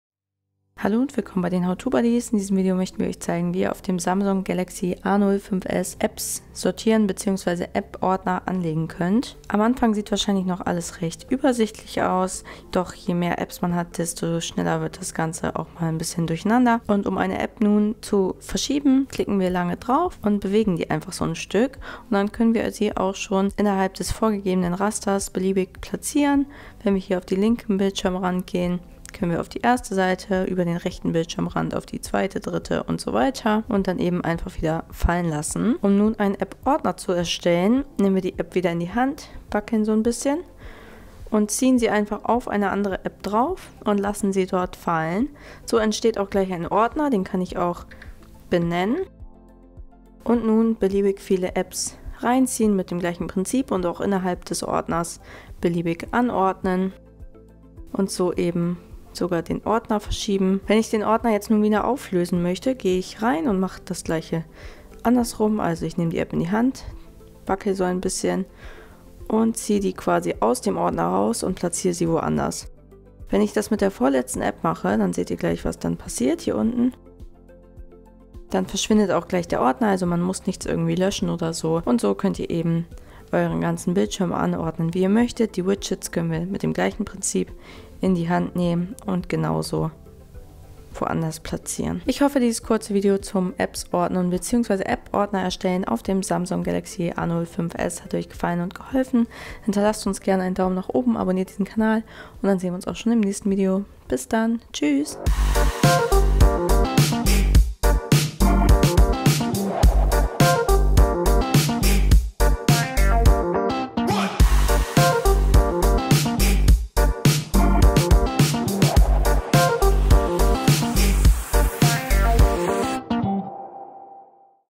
Samsung Galaxy A05s App Ordner erstellen Apps sortieren
Ihr wollt auf Eurem Samsung Galaxy A05S einen App-Ordner erstellen, um mehrere Apps zu bündeln und damit Platz einzusparen? Wir zeigen Euch, wie das Platzsparen auf dem A05S funktioniert und wie man z.B. Apps der gleichen Kategorie in einen Ordner bewegen kann. Damit braucht Ihr nicht so viel hin und her zu swipen und habt Apps der gleichen Kategorie immer gut sortiert und schnell parat. Denn bei den vielen Apps, die man ja heute nutzt, verliert man schonmal gern den Überblick und muss oft lange suchen, um eine bestimmte Anwendung zu finden. Auch zeigen wir Euch, wie Ihr einen Ordner wieder auflöst und generell Apps über Euer Display bewegt. Viel Spaß beim Erstellen Eurer eigenen Ordner mit dem neuen Samsung Galaxy A05S.